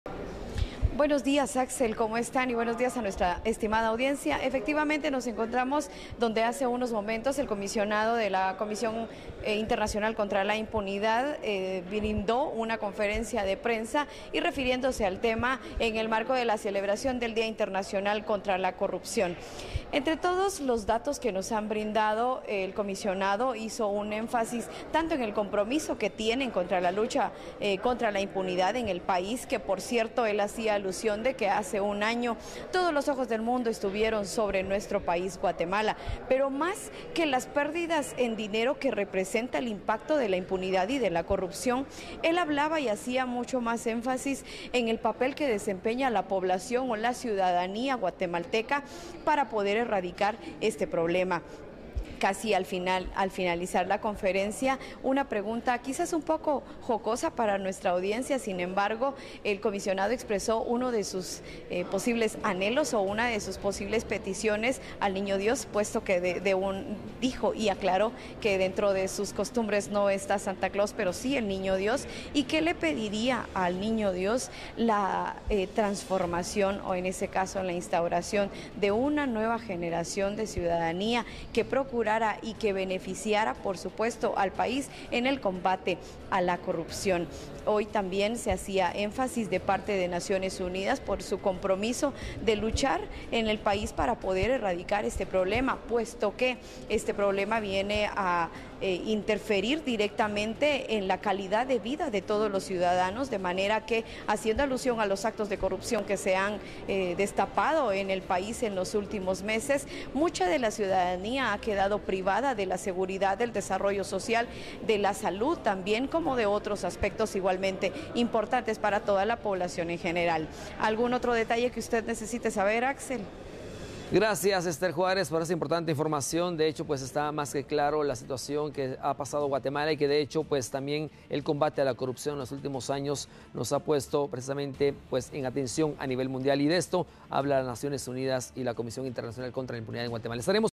Hoy se celebra el Día Internacional Contra la Corrupción
La información fue entregada por Rebeca Farías, Coordinadora Residente de la ONU, quien habló sobre el trabajo realizado en el país.

La CICIG y Naciones Unidas se refieren a la situación en Guatemala